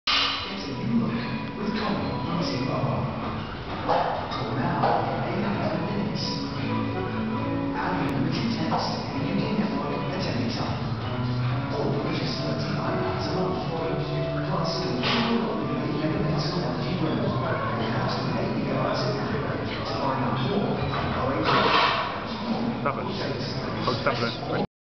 angie mc namara single dumbell rows 65lbs limerick ireland
training journal started weightlifting on 0ct 3rd and i am now two months in,36 weks out for competition